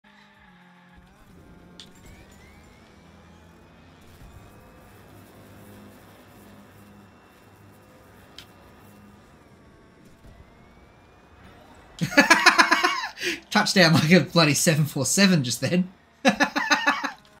And we have Touchdown!
Just a clip from a little warmup to take photos before World Race.